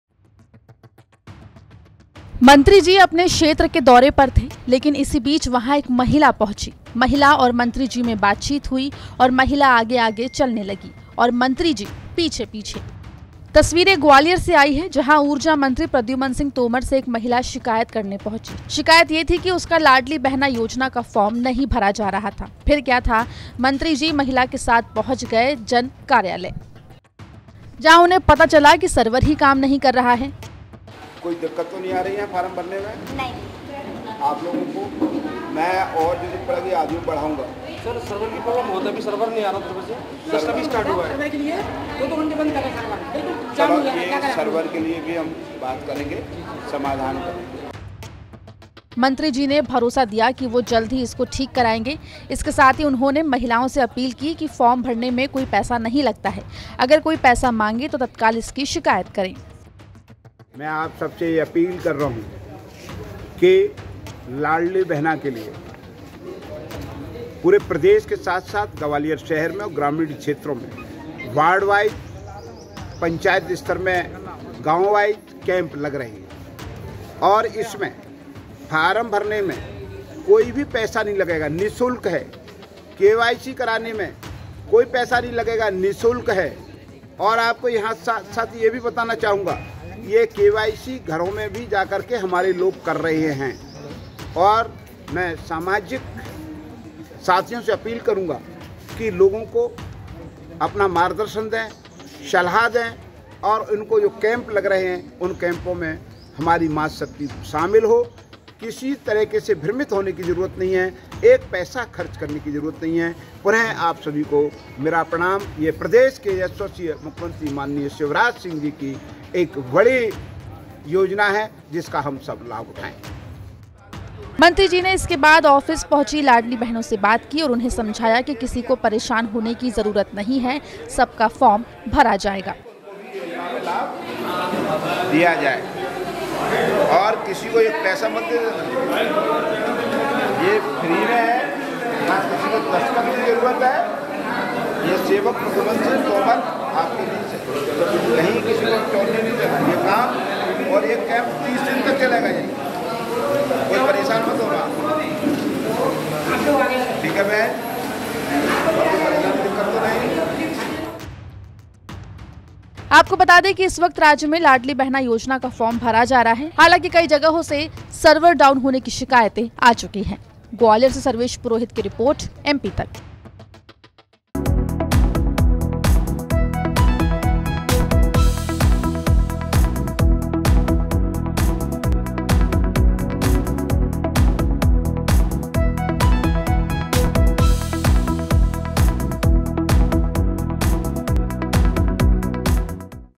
आगे-आगे चल रही थी महिला, पीछे-पीछे मंत्री जी, जिसने देखा वो हैरान हो गया ! | MP Tak
Gwalior में उर्जा मंत्री Praduman Singh Tomar का अलग अंदाज देखने को मिला है. जहां महिला ने मंत्री जी से लाडली बहना योजना का फार्म न भरने को लेकर शिकायत की है.फिर क्या था मंत्री जी महिला के साथ जन कार्यालय पहुंच गए. और वहां का जायजा लिया. इस दौरान मंत्रीजी ने कहा कि सर्वर जल्द ठीक करने और महिला का फार्म भरने का भरोसा दिया.